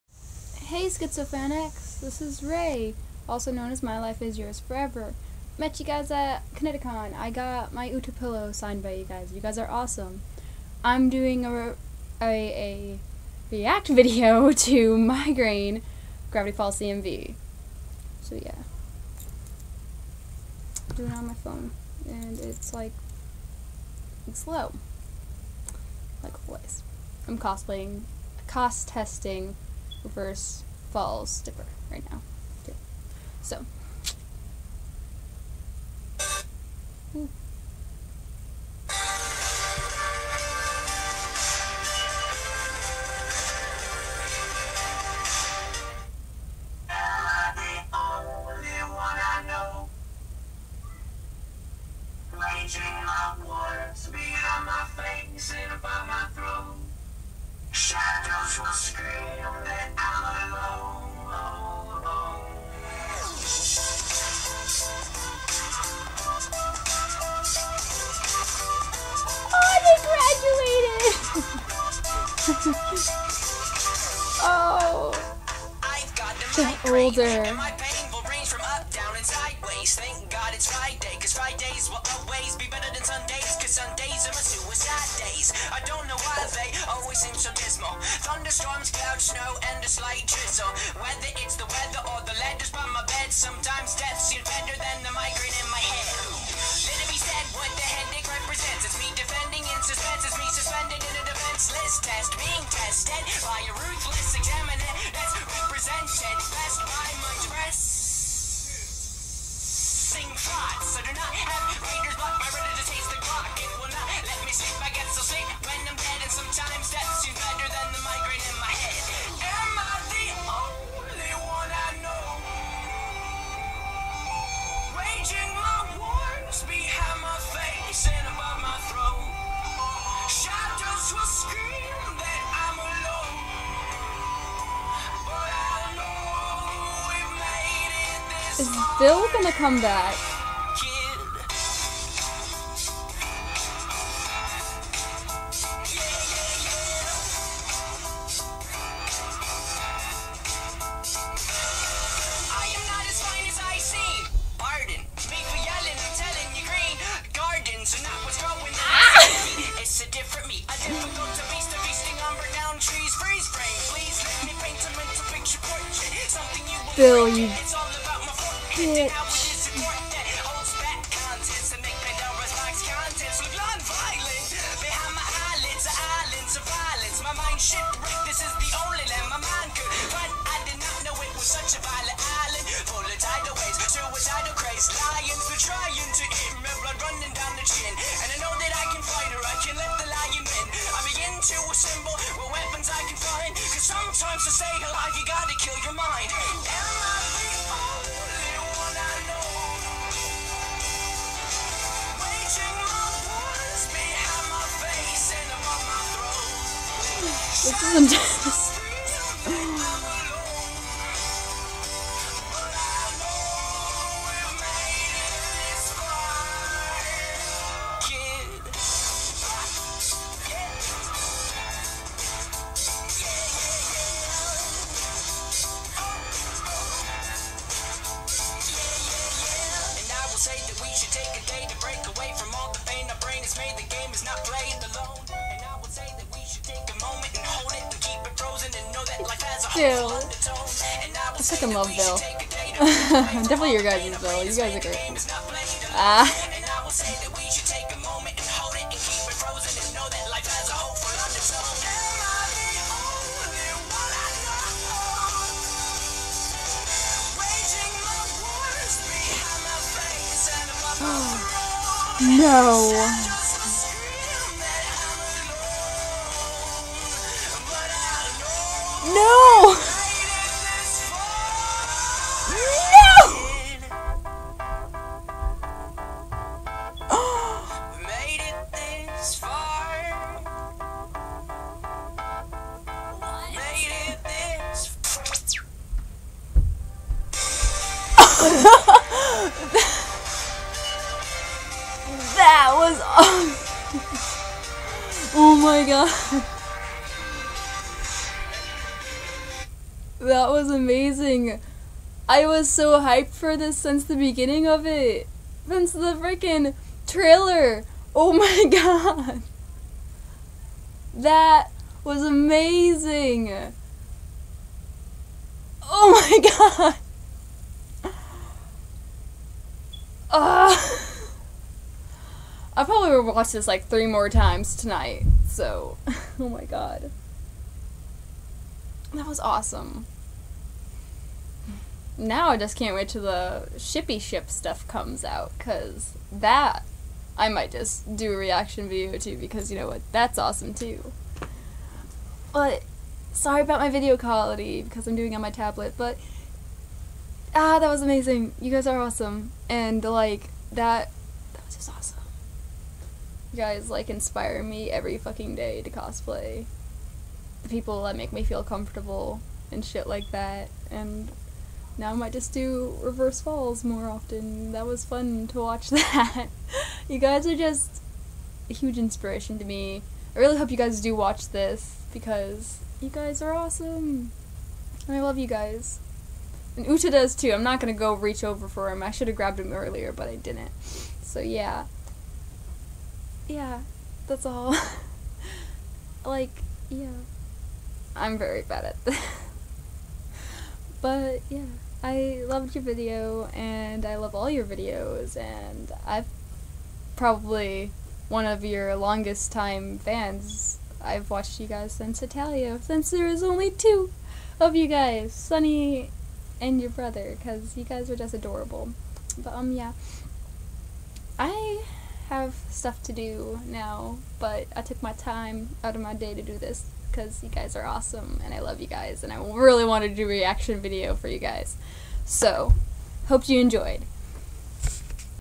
SkitsoFanActs reaction video: Migraine
Sorry for my poor camera quality but you guys inspire me everyday to cosplay people that make me feel comfertable and that i love no matter what others say!  Ive been watching for 4 years sense the begining and i must say you guys improved drasticly in videos over the years i watched a real old video and i loved it and watching this reminded me 'this could be me in 4 years' so thank you guys your awesome and i hope you guys injoyed.